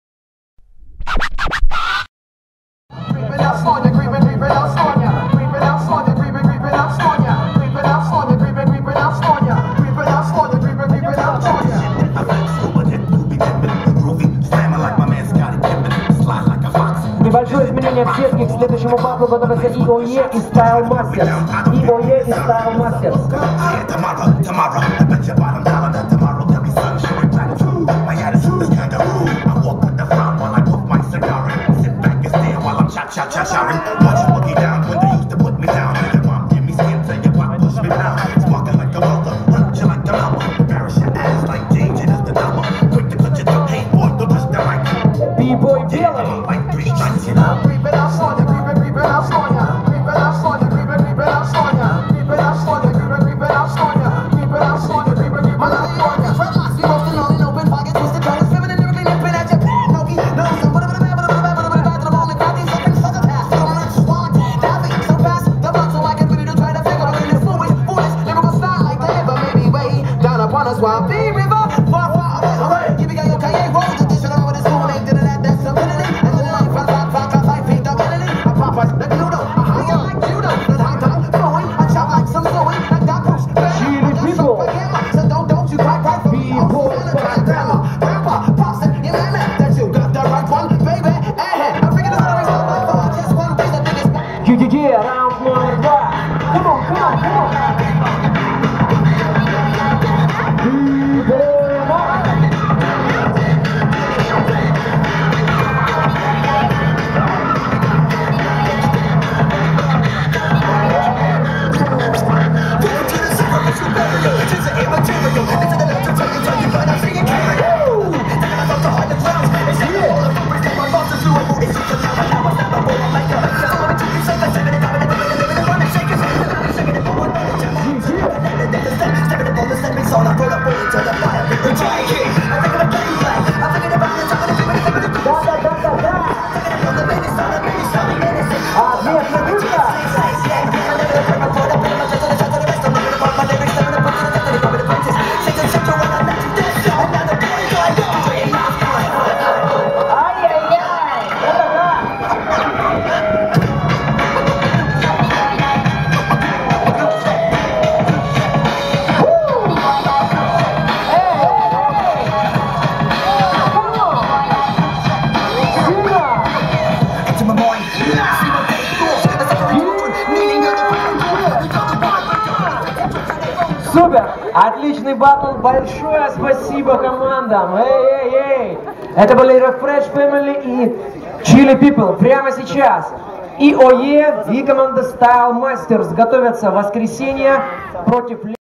Refresh Family vs Chelle people - MOVE - START UP 2 (2014) - Break Dance pro 1x1
30 мая - 1 июня 2014г.
г. Ростов-на-Дону